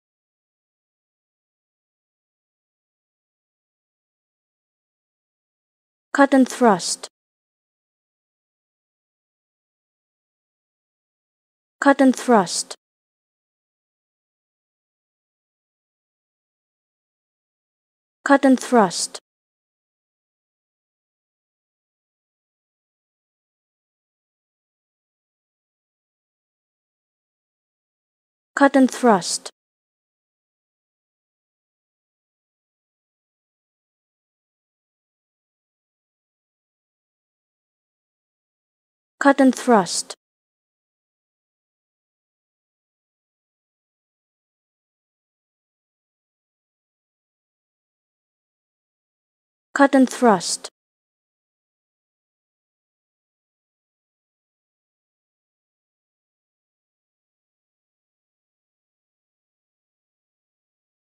How To Say Cut-And-Thrust
Learn how to say Cut-And-Thrust with EmmaSaying free pronunciation tutorials.